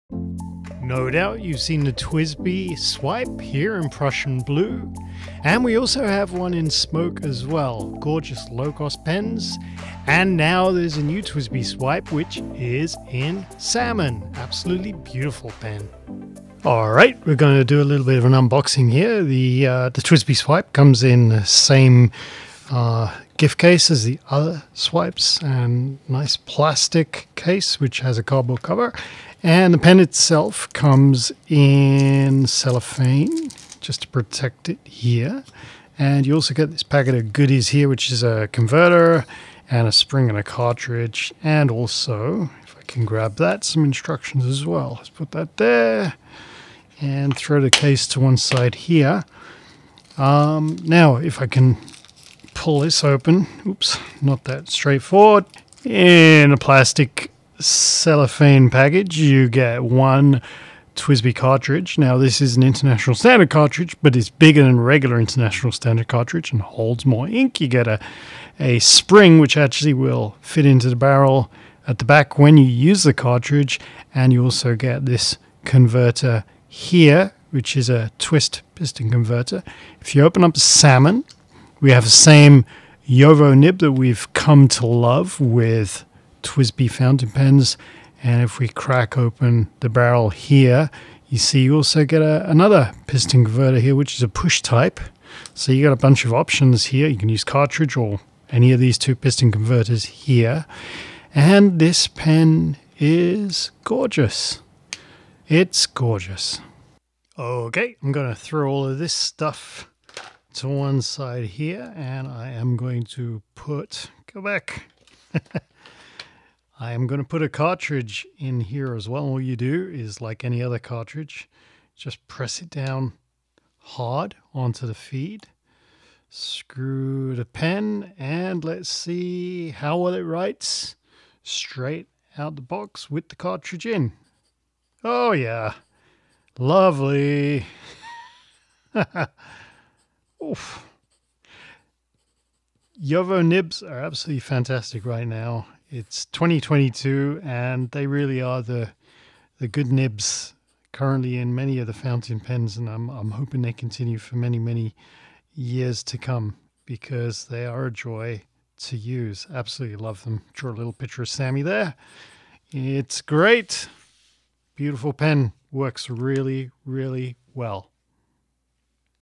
TWSBI Salmon Swipe Quick Peek
Video # 19 is a quick peak of the TWSBI Swipe in a lovely salmon color! We now have the Prussian Blue, the Smoke and this gorgeous color to add to the wonderful Swipe collection.

0:16 - Unboxing the TWSBI Swipe Salmon.
0:55 - The converters and cartridge.
1:46 - Inserting the cartridge.
2:13 - The writing test.